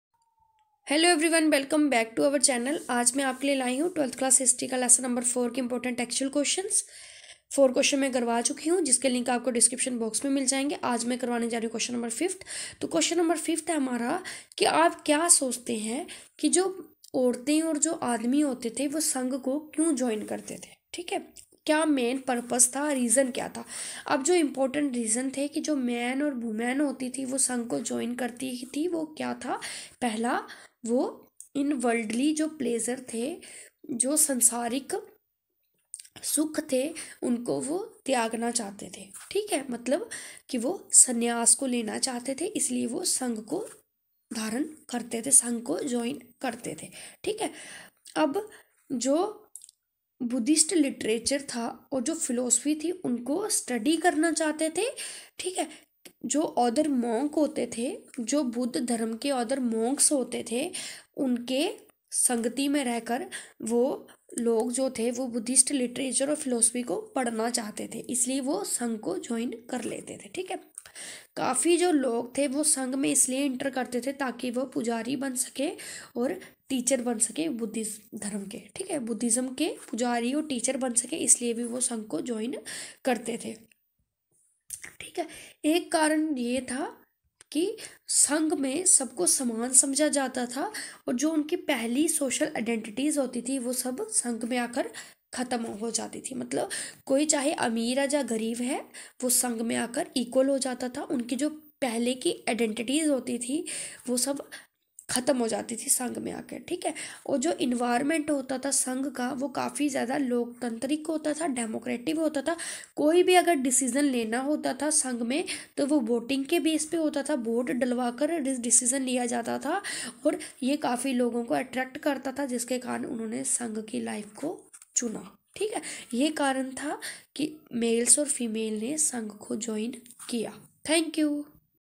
Q.5. Why do you think women and men joined the sangha?
12th History Chapter 4

Thinkers Beliefs and Buildings

Important Textual Questions